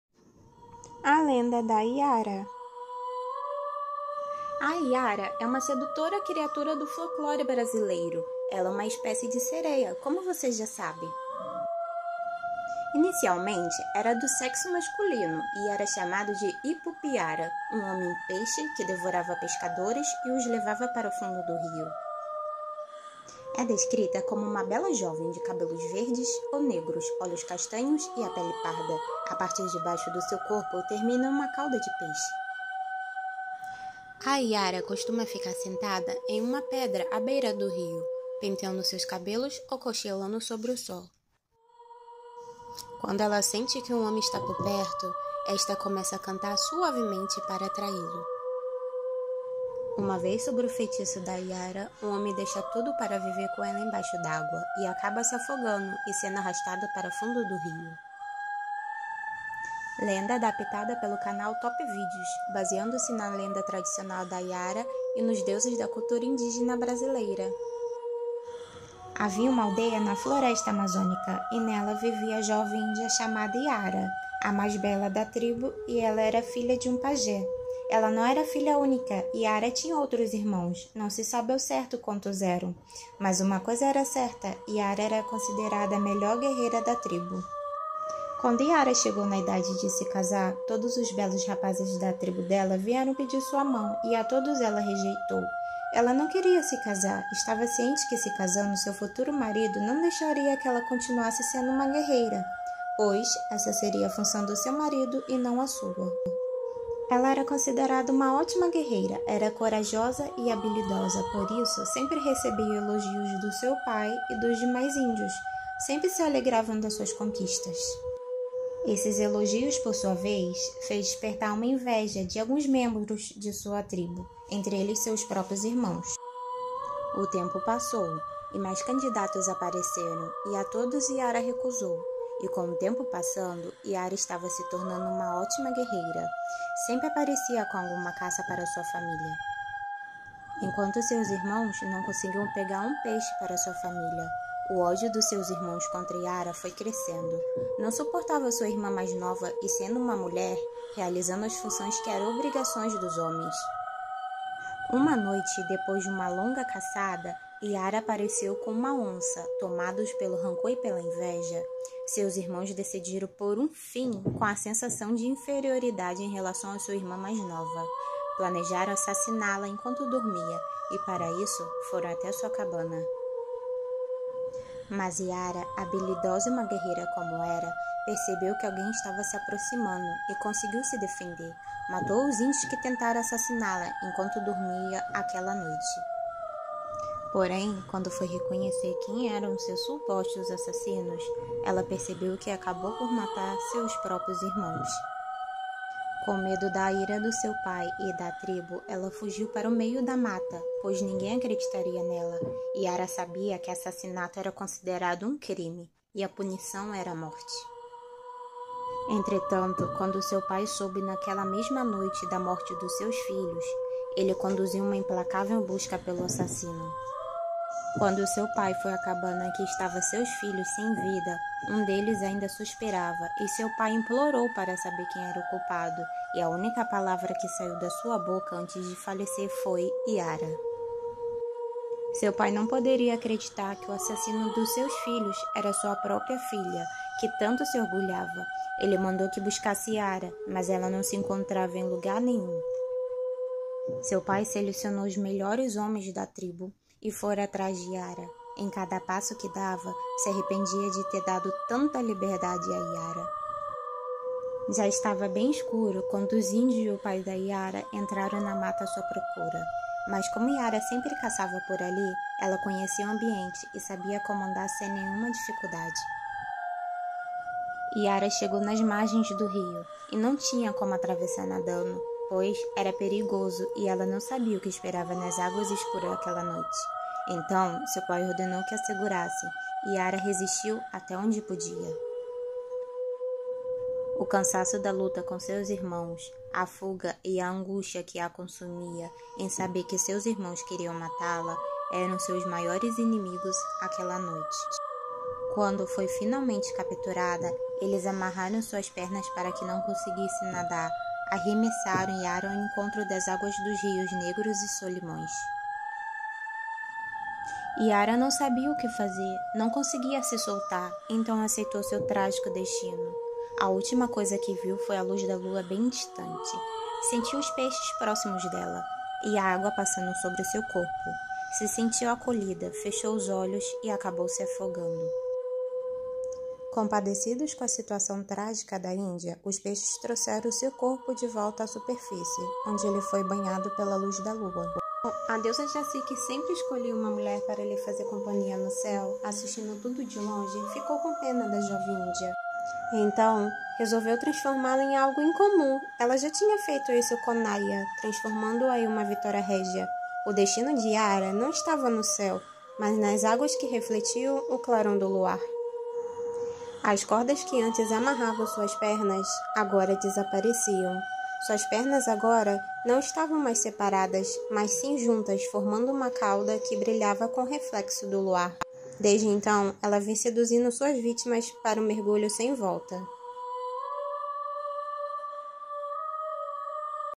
A LENDA DA IARA ( história narrada)
A Iara é uma sedutora criatura do folclore brasileiro. Ela é uma espécie de sereia como você já sabe.

Inicialmente era do sexo masculino e era chamado de Ipupiara, um homem peixe que devorava pescadores e os levava para o fundo do rio.
históriadeterror